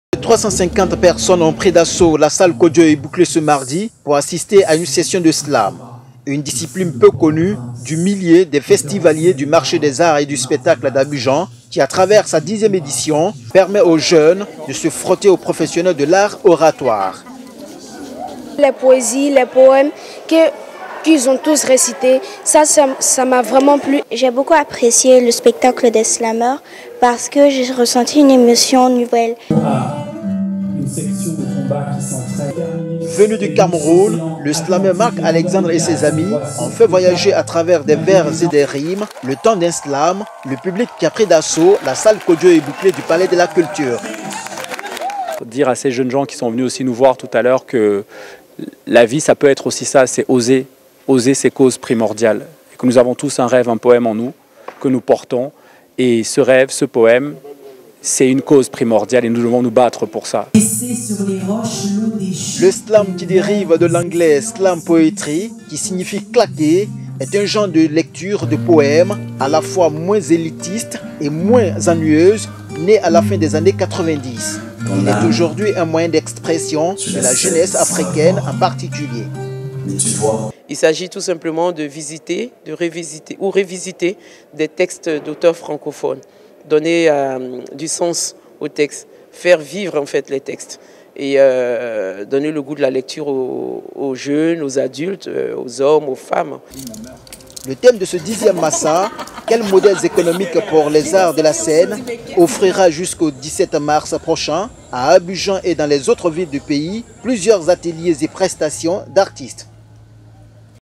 Capitaine Alexandre - Le slam fait escale à Abidjan
Reportage sur la semaine de slam organisée à l'occasion du "Festival du marché des arts et du spectacle d'Abidjan" qui s'est tenu du 16 au 17 mars 2018.